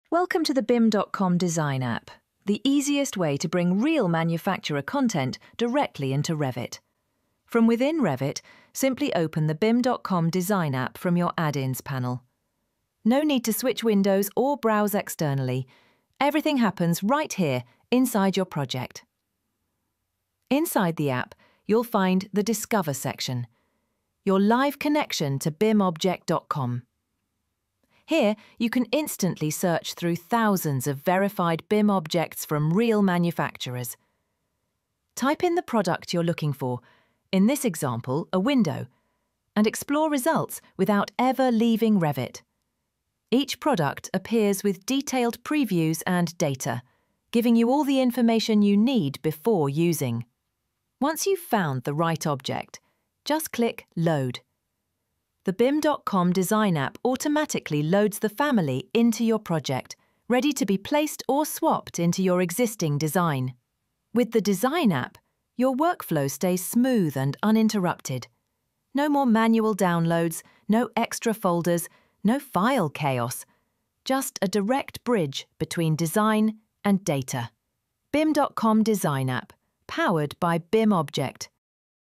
How to use the Bim.com Design App in Revit | Seamless BIMobject Integration
Bring real manufacturer content straight into Revit - no downloads, no switching windows, no file chaos.
With the Bim.com Design App, you can search thousands of verified BIM objects from real manufacturers and load them directly into your project, all without leaving Revit.